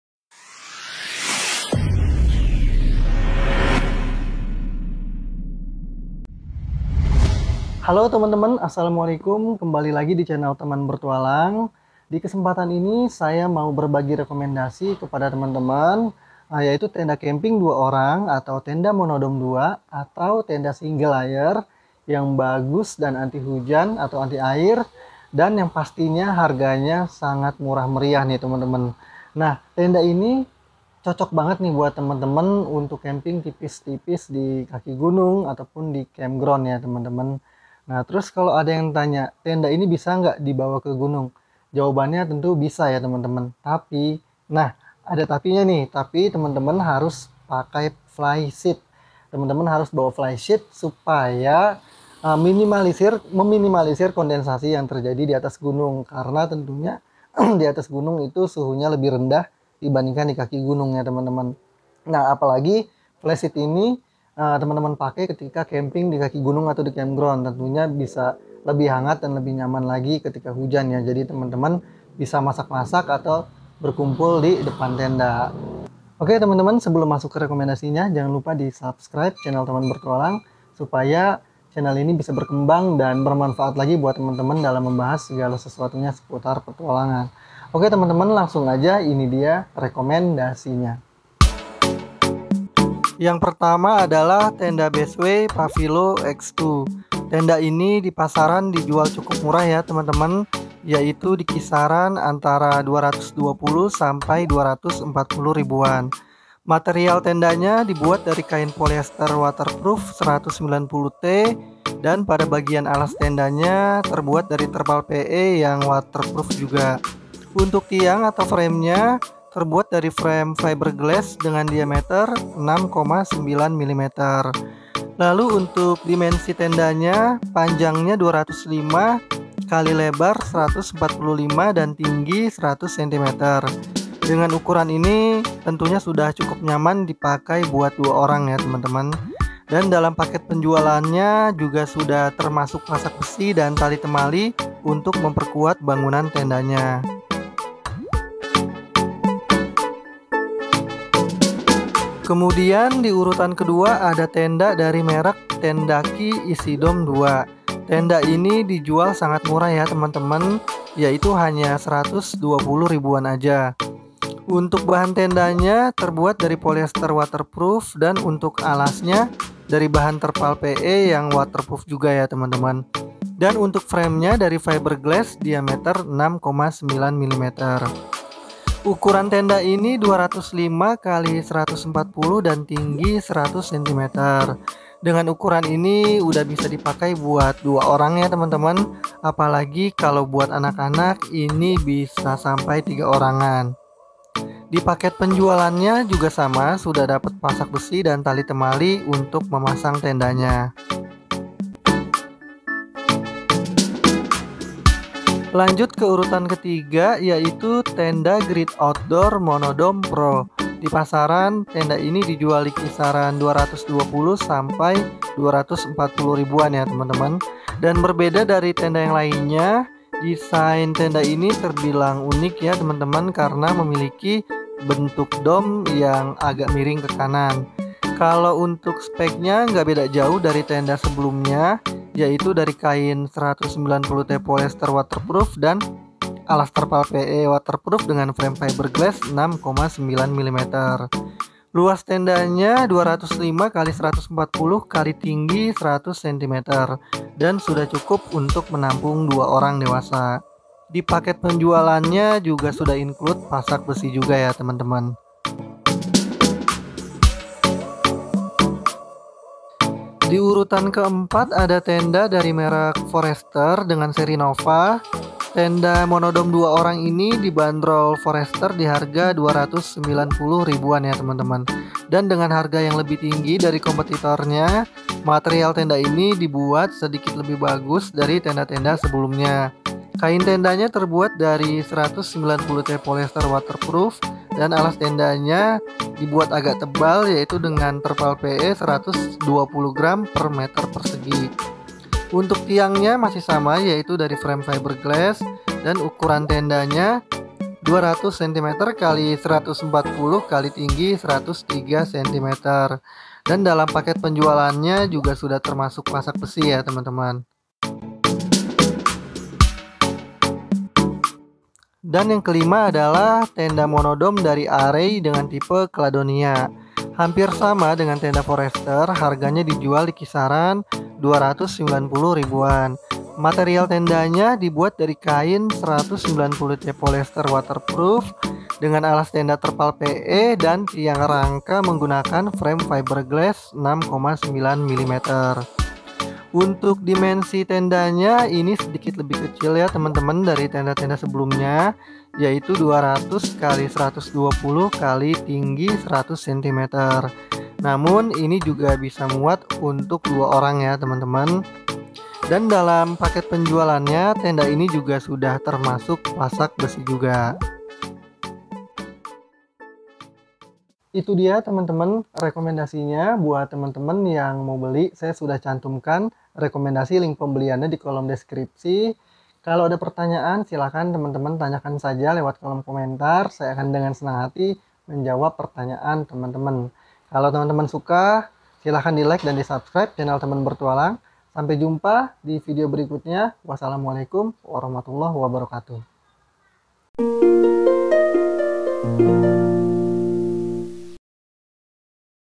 TENDA CAMPING 2 ORANG Murah Meriah | Tenda camping anti hujan
kali ini saya akan ngebahas tenda camping 2 orang murah yang sudah anti air dan dapat dipakai untuk outdoor.

Tenda monodome  ini akan lebih maksimal lagi fungsinya jika ditambah flysheet pd lapisan luarnya sehingga bisa juga dipakai buat ke gunung sekalipun.

* Tendaki easy dome 2.

* Great outdoor monodome pro 2p.

* Tenda Forester nova.

* Tenda Arei Cladonia.

Tambahan :
FLYSHEET